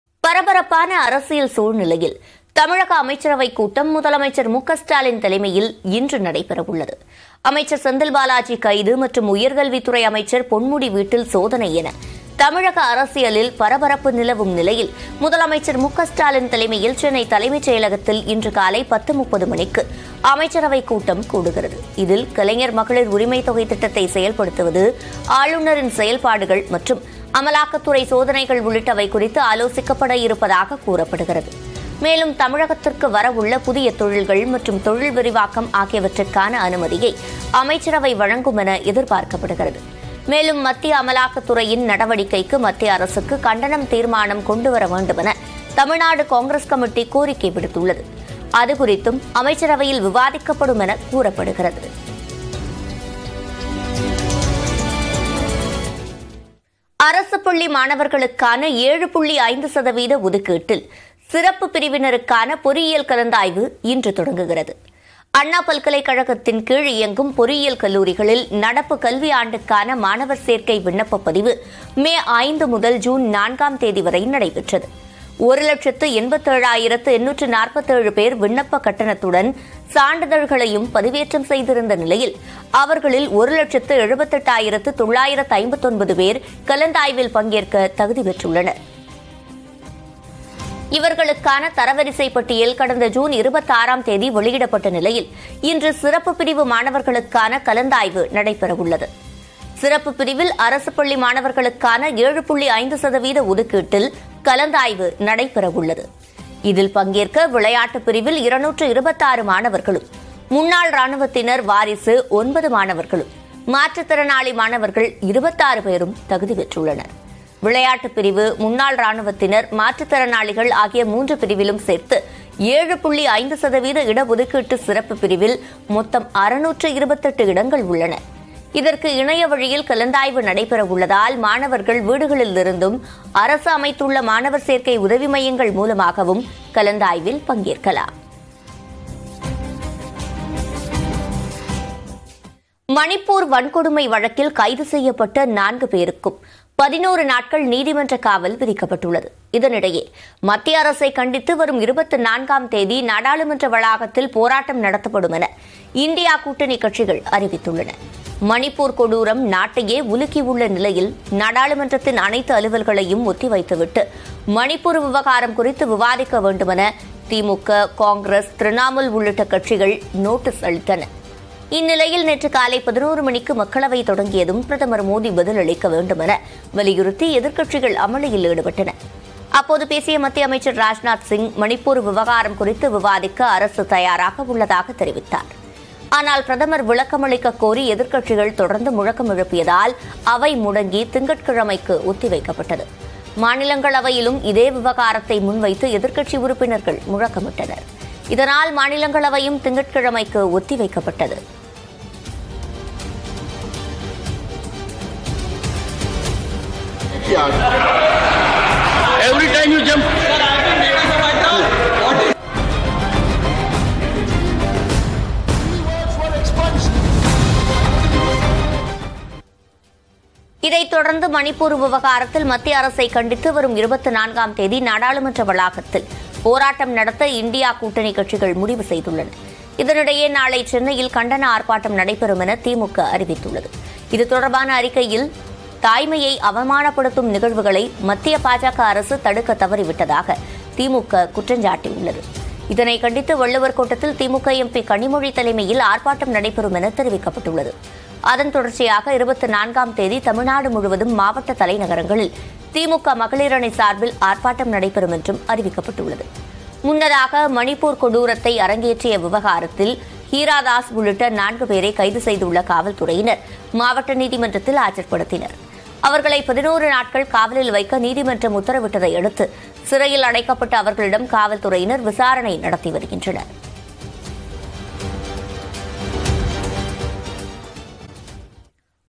முதலமைச்சர் தலைமையில் அமைச்சரவை கூட்டம் - ஆளுநர், அமலாக்கத்துறை குறித்து விவாதம்? | MK Stalin
முதலமைச்சர் தலைமையில் அமைச்சரவை கூட்டம் - ஆளுநர், அமலாக்கத்துறை குறித்து விவாதம்?